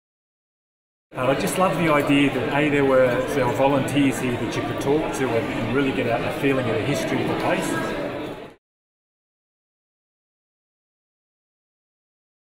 My WarMuseum10 story - Raymond Bernstein
In celebration of the War Museum's 10th anniversary on LeBreton Flats, we asked visitors to tell us what the Museum meant to them.

To explore other people’s stories, or to submit your own, please